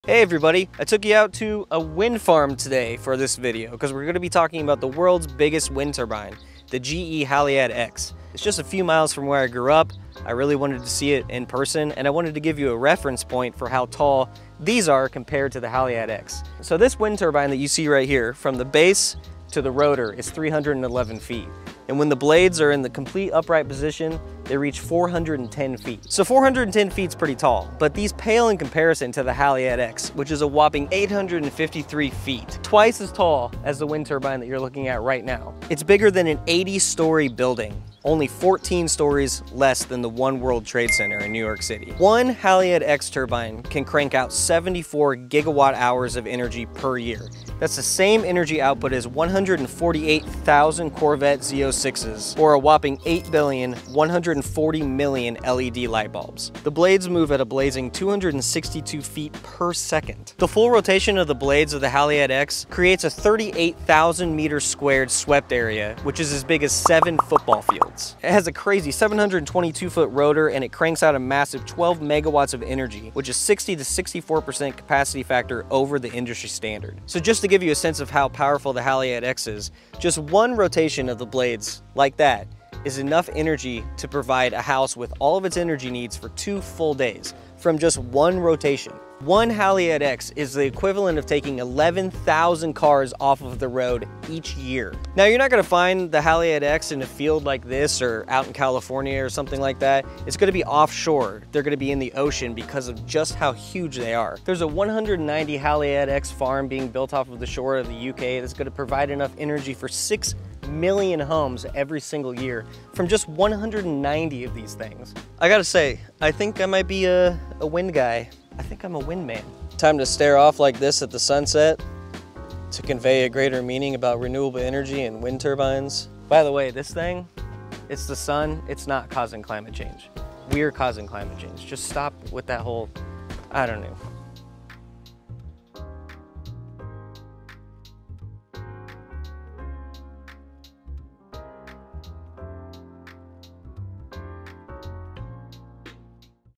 World's Biggest Wind Turbine: The GE Haliade-X
If you think the wind turbines you drive by on the interstate are big, you haven't met GE's Haliade-X which puts them all to shame. Check out this quick video for all stats on the Haliade-X including the fact that this thing has a 38,000 m2 swept area which is as big as 7 FOOTBALL FIELDS.

We give away 50% of everything we make to climate relief efforts.